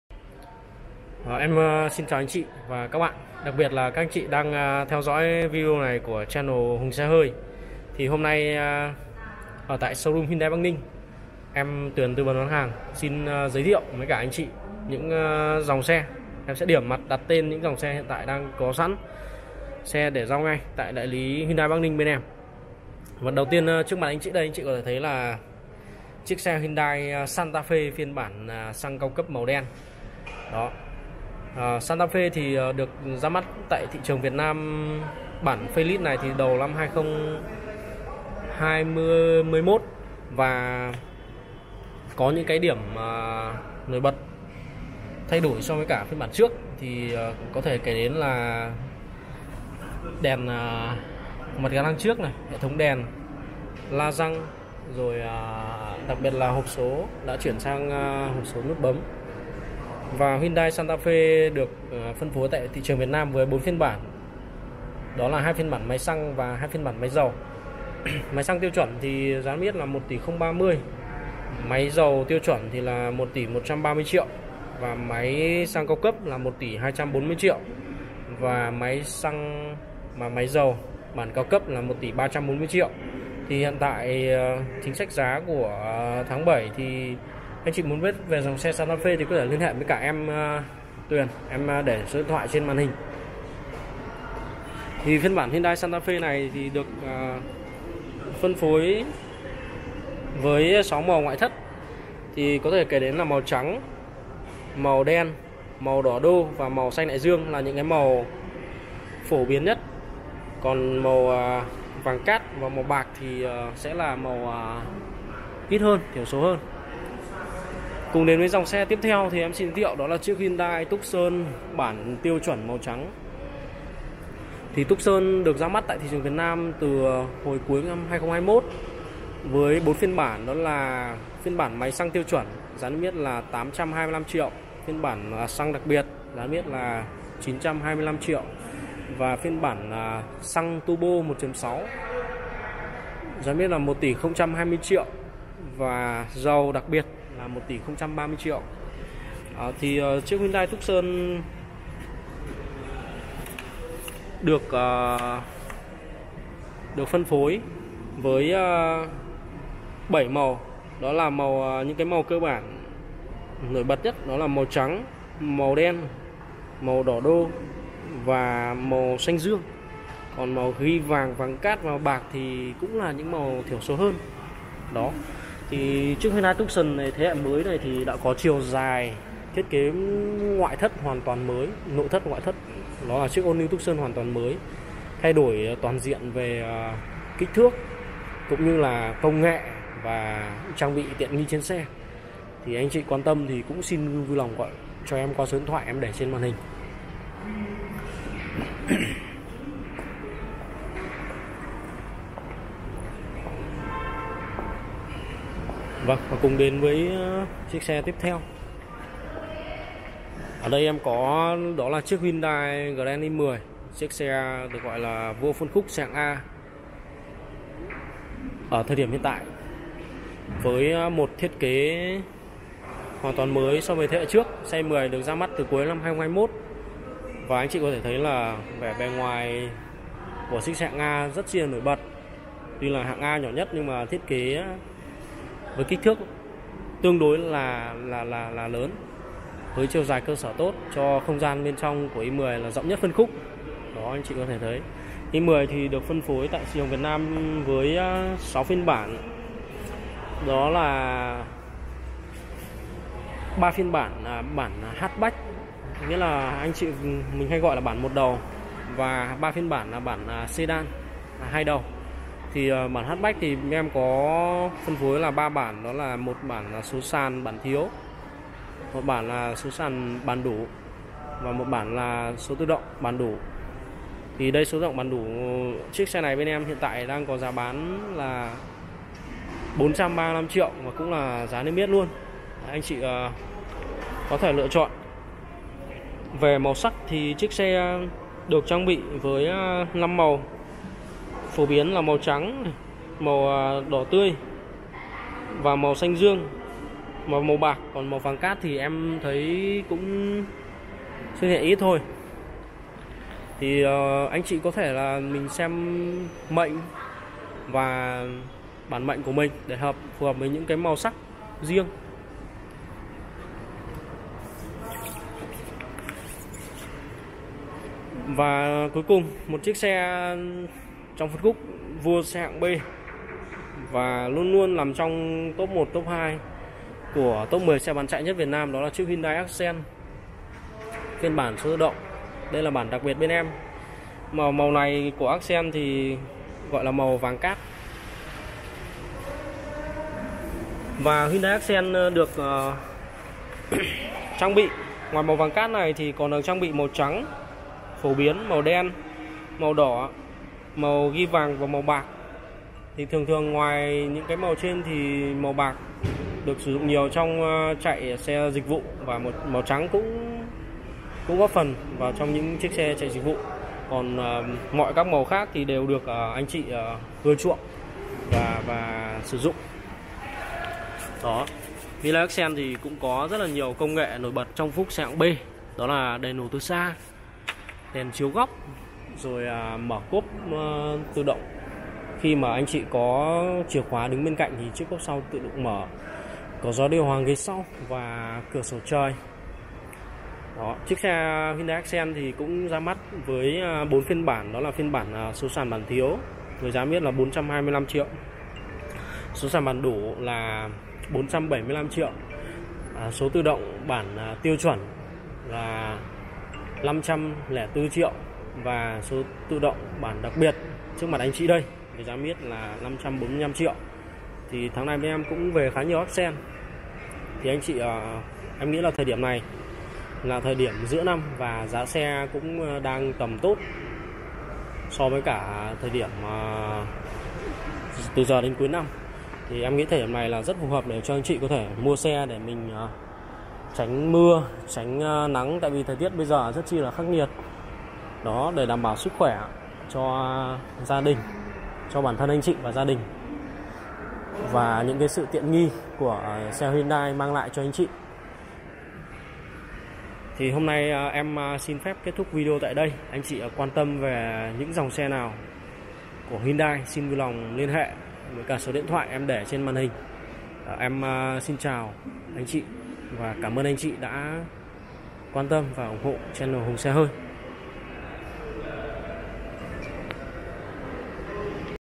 Giá Xe Hyundai Tháng 7-2022 Tại Bắc Ninh | Giá Lăn Bánh Mới Nhất Kèm Khuyến Mại Tucson, Santafe,..
+ Hotline Tư Vấn: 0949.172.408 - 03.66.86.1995